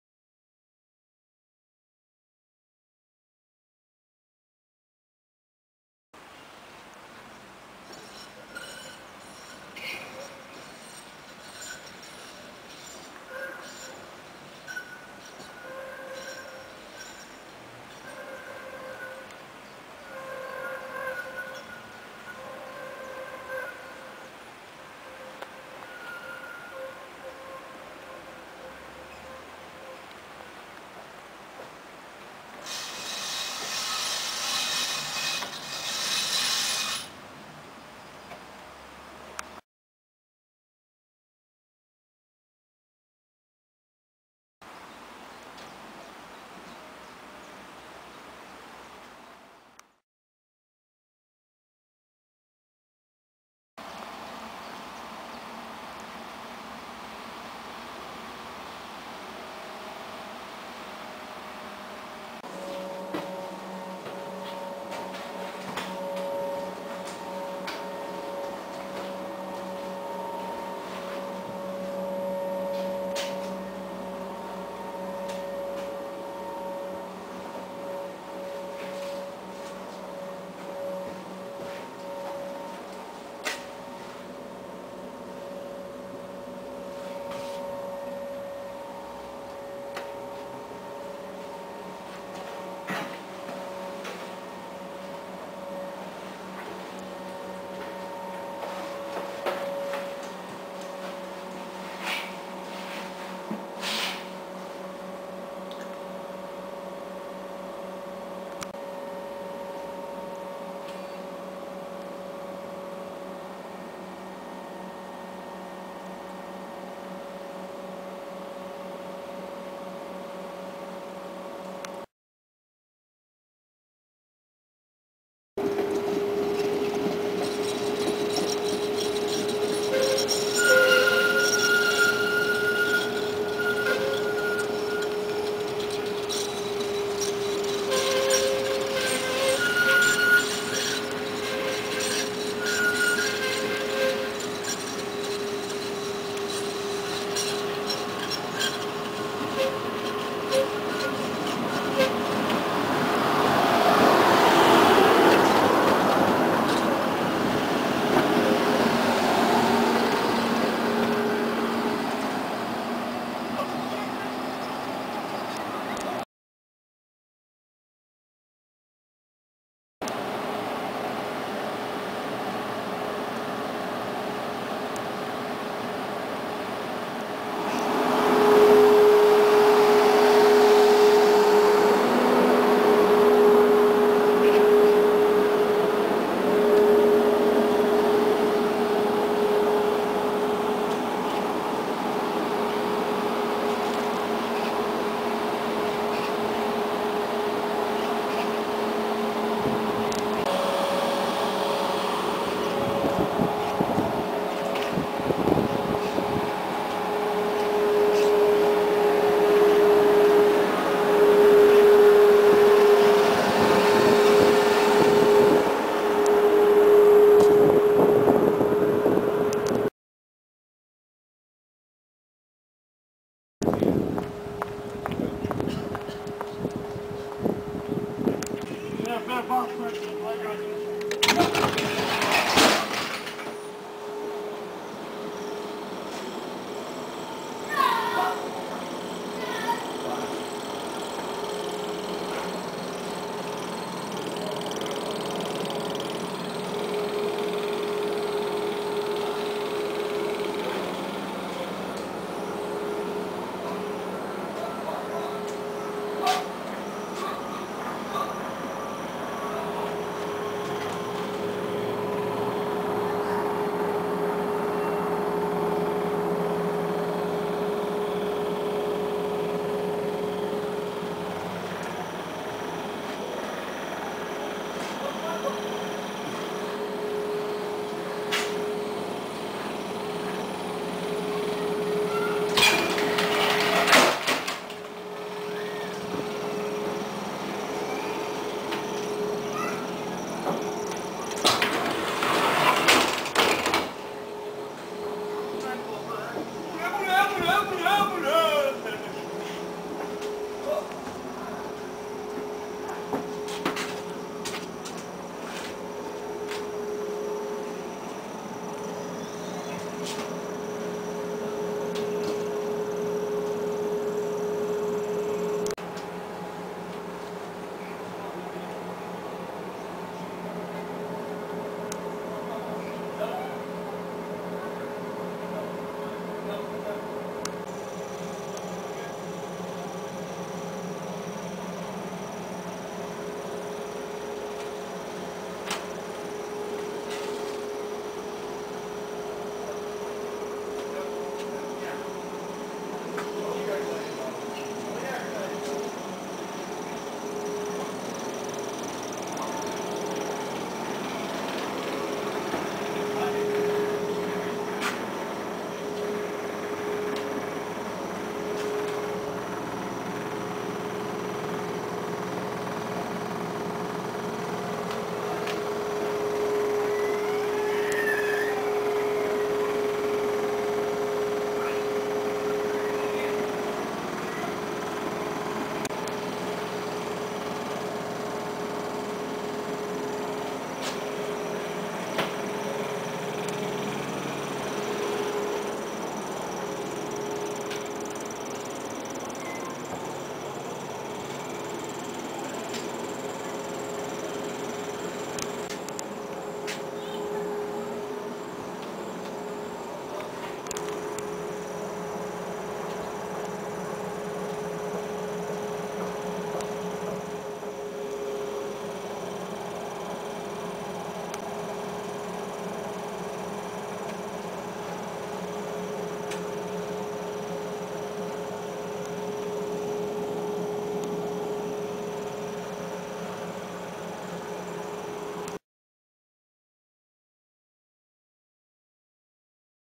Kraków - Linia 17 - Konstal 105Na - 488+427 - Zajezdnia Nowa Huta -
Prezentacja składu Konstal 105Na - 488+427
Zajezdnia Nowa Huta - brygada 17-02
Linia 17 -
Kopiec Wandy - Os. Piastów
Os. Piastów - Kopiec Wandy